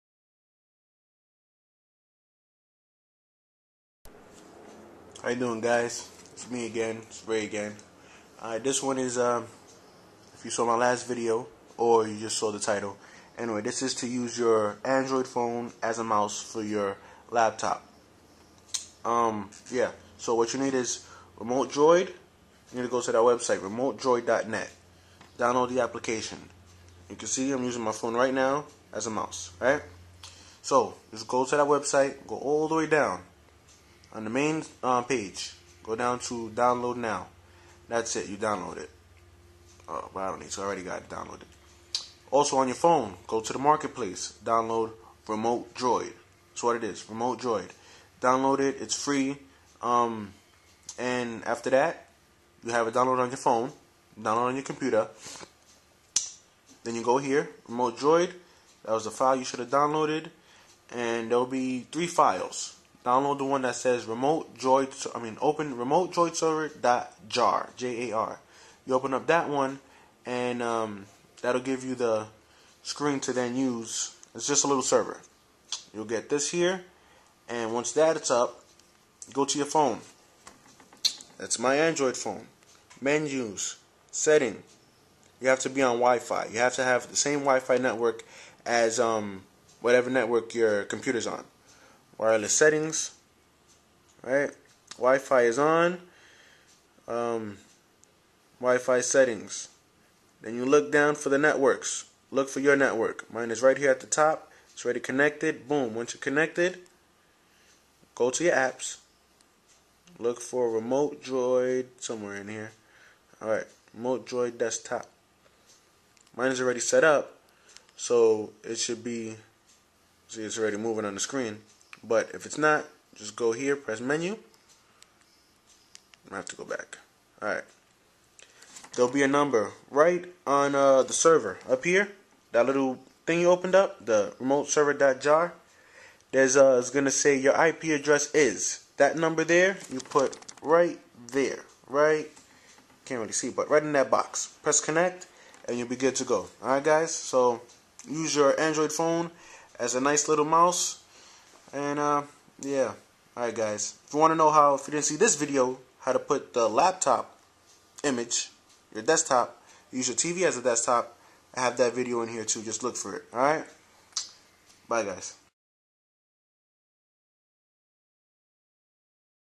Use your android phone as wireless mouse
Use your android phone as a wireless mouse, I show you the app